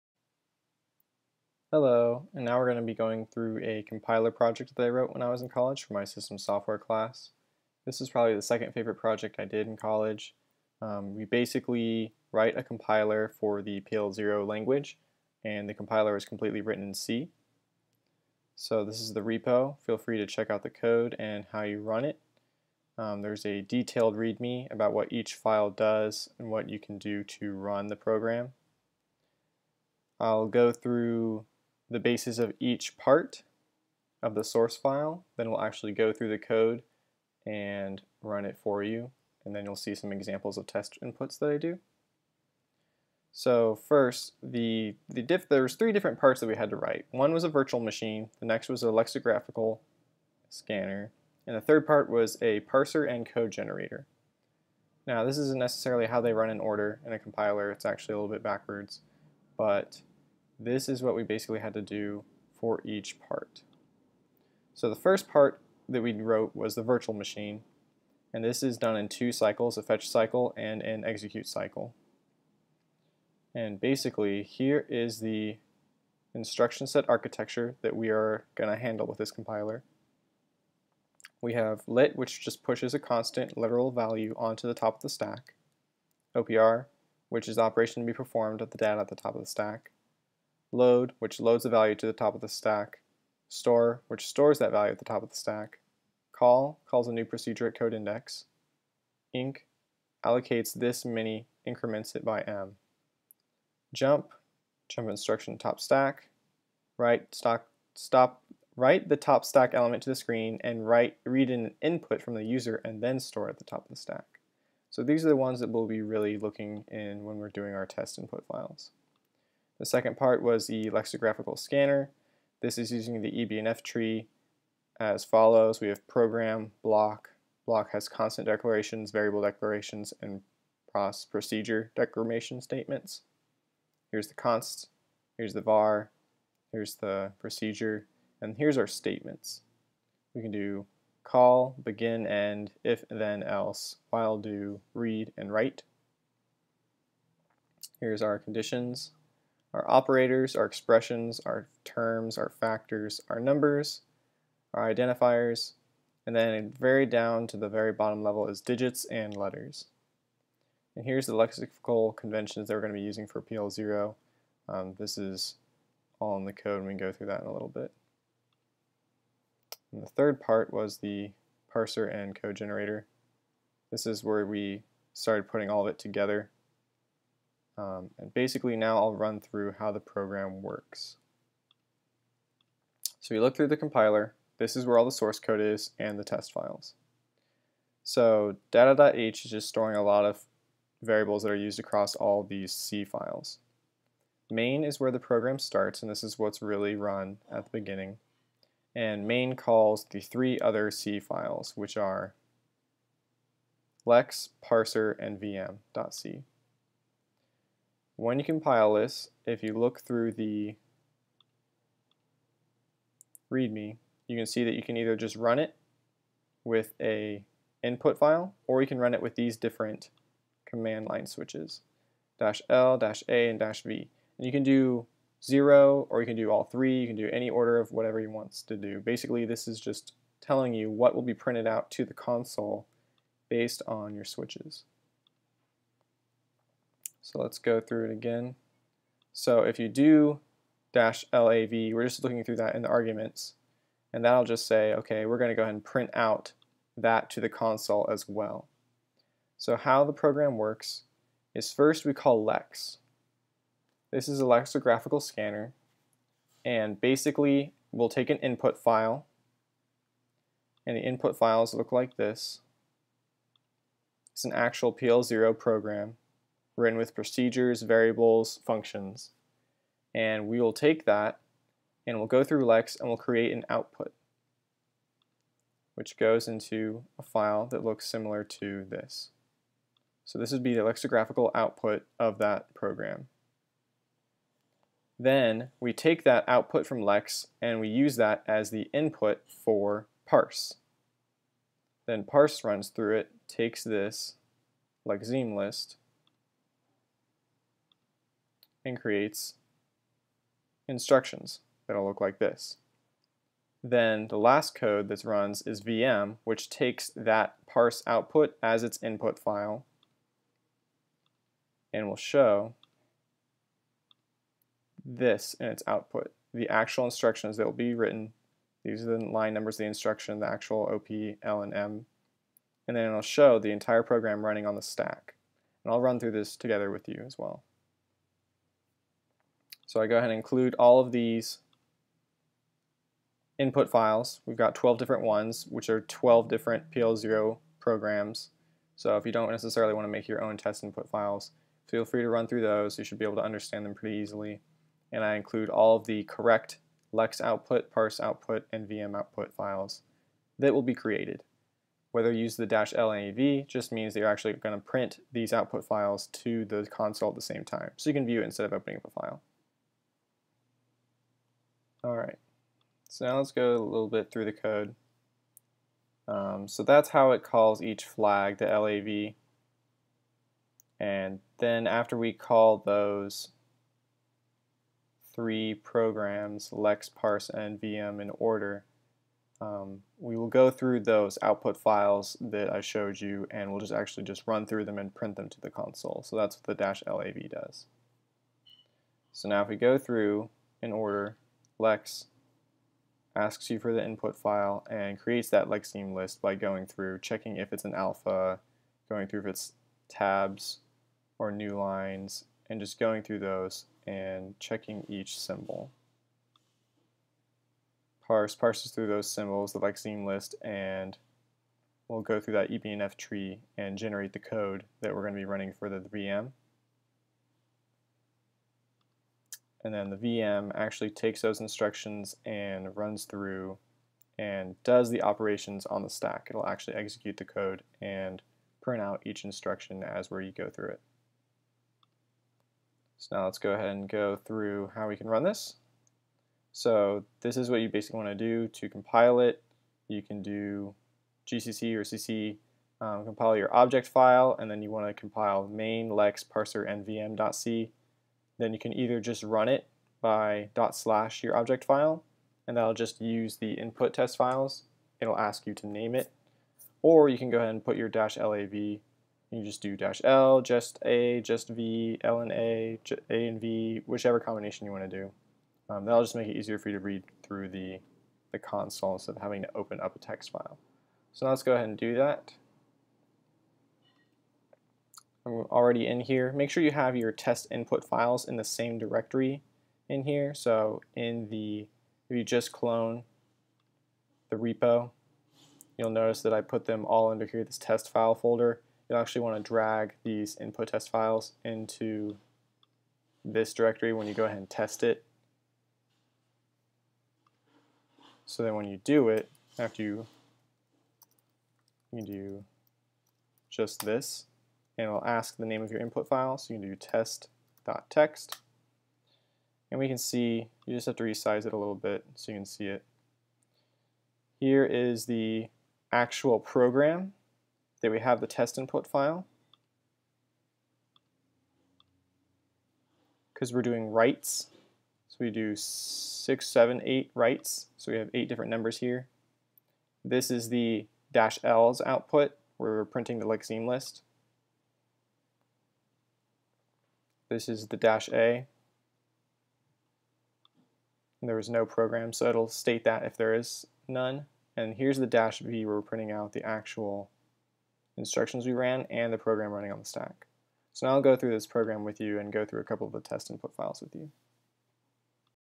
PL/0 Compiler: Introduction and Code [1/2]
Compiler for the PL/0 programming language, written entirely in C. This was a fun project I did in college, and really highlights the fundamentals of how a compiled language executes on the stack.

This video goes over the information behind the project, how it works, the code, and how to run the project.